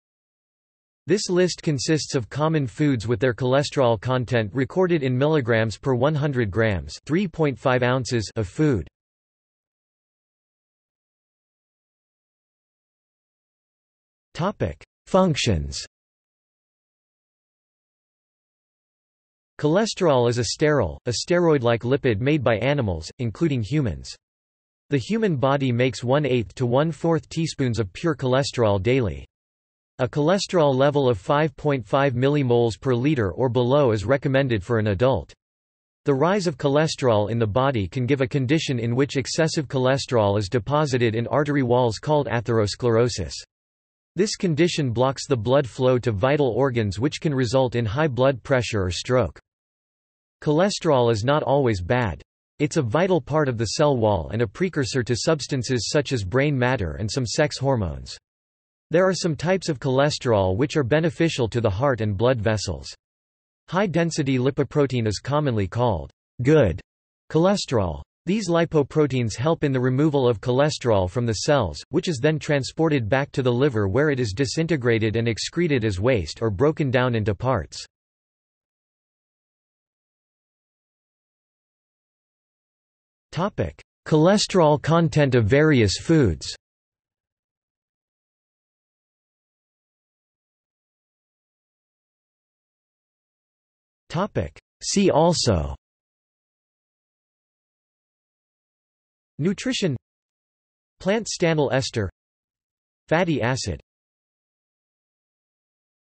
List of cholesterol in foods | Wikipedia audio article
This is an audio version of the Wikipedia Article:
List of cholesterol in foods

00:00:10 1 Functions
00:01:19 2 Cholesterol content of various foods
00:01:30 3 See also

- Socrates

SUMMARY
This list consists of common foods with their cholesterol content recorded in milligrams per 100 grams (3.5 ounces) of food.